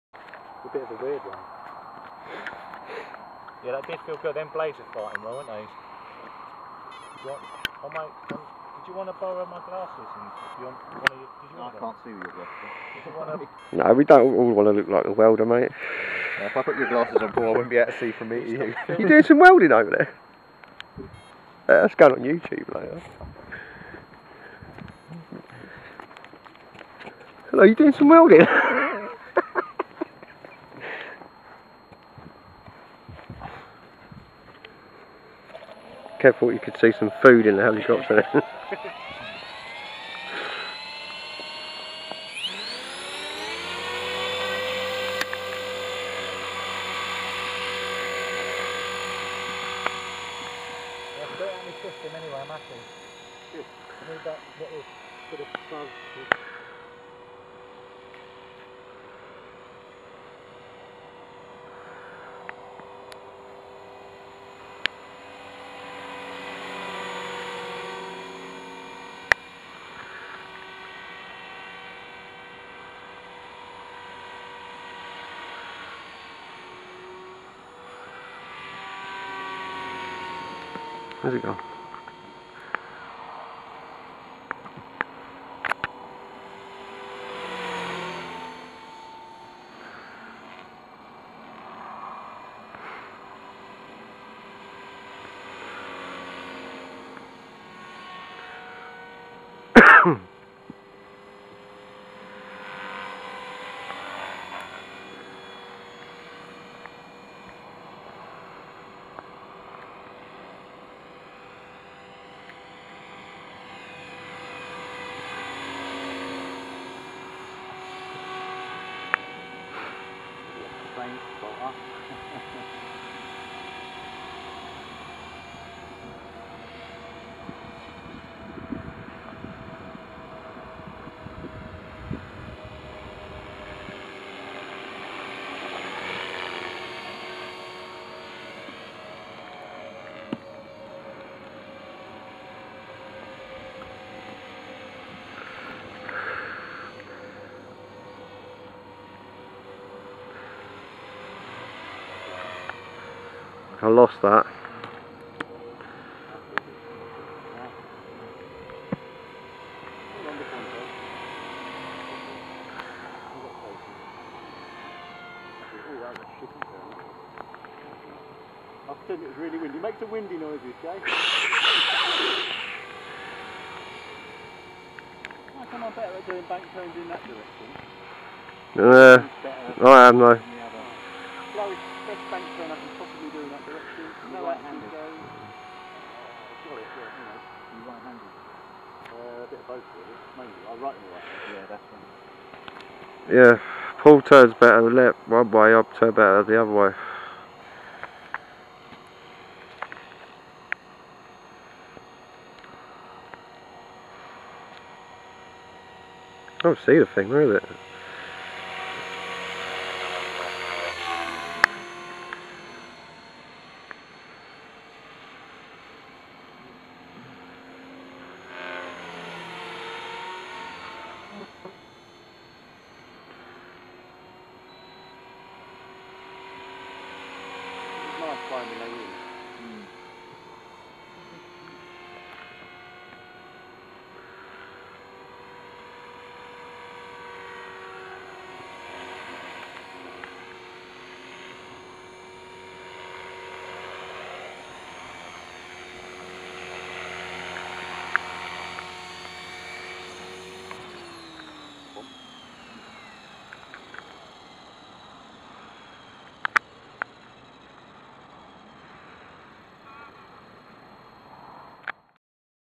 The Align T-rex 450 pro -Staring Mushey- oh and The welder lol
mush , pec and steamy playing T-rex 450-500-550
this is mush77 flight with his Align T-rex 450 Pro